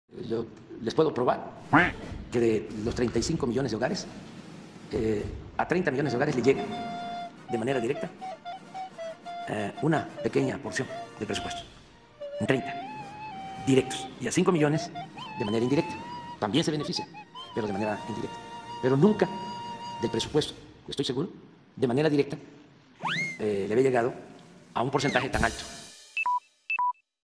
Les puedo probar que a 30 millones de hogares les llega presupuesto de manera directa: AMLO® 🦆🤥
Les puedo probar que de los 35 millones de hogares, a 30 millones de hogares les llega una pequeña porción del presupuesto de manera directa: AMLO® 🦆🤥